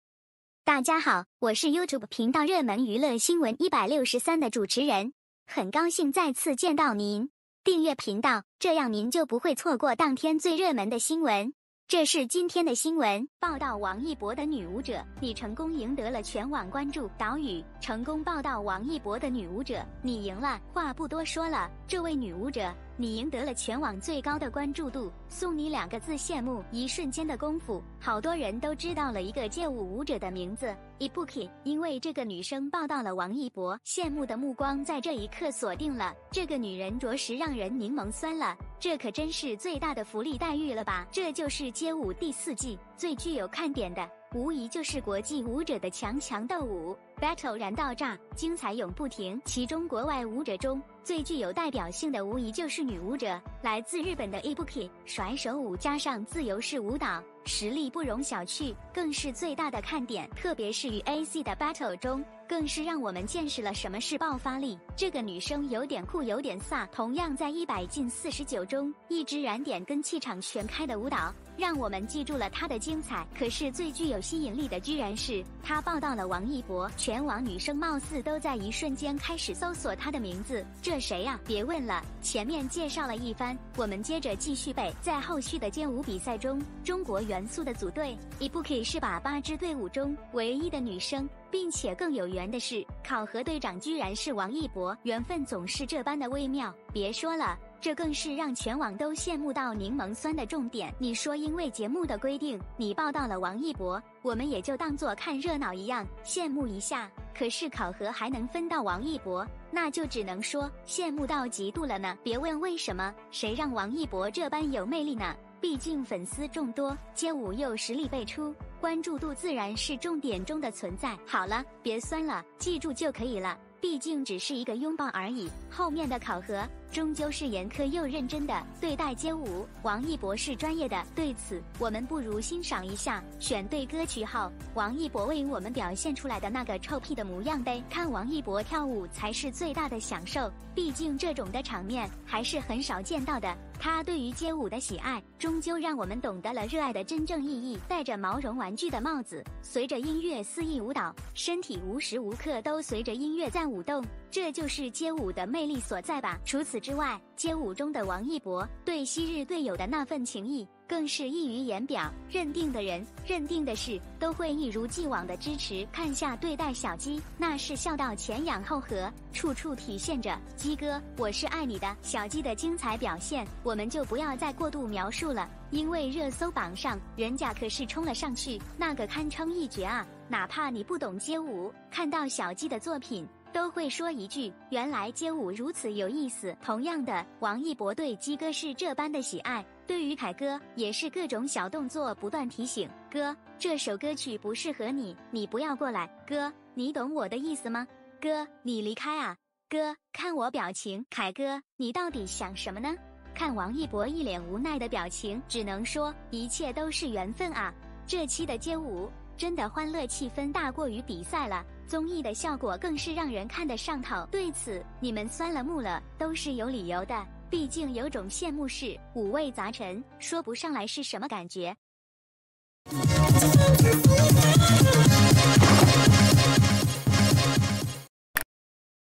抱到王一博的女舞者，你成功赢得了全网关注！. | 王一博 - 热门娱乐新闻163
热门娱乐新闻163 05/09/21: 抱到王一博的女舞者，你成功赢得了全网关注！. :  抱到王一博的女舞者，你成功赢得了全网关注！.  导语：成功抱到王一博的女舞者，你赢了！  话不多说了，这位女舞者，  你赢得了全网最高的关注度，送你两个字"羡慕"！  一瞬间的功夫，好多人都知道了一个街舞舞者的名字，  ibuki，因为这个女生抱到了王一博！  羡慕的目光在这一刻锁定了，这个女人着实让人柠檬酸了，  这可真是最大的福利待遇了吧。  《这就是街舞》第四季，最具有看点的，  无疑就是国际舞者的强强斗舞，  battle燃到炸，精彩永不停。  其中国外舞者中，最具有代表性的无疑就是女舞者，  来自日本的ibuki，甩手舞加上自由式舞蹈，  实力不容小觑，更是最大的看点。  特别是与AC的battle中，  更是让我们见识了什么是爆发力，这个女生有点酷有点飒！  同样在100进49中，一支燃点跟气场全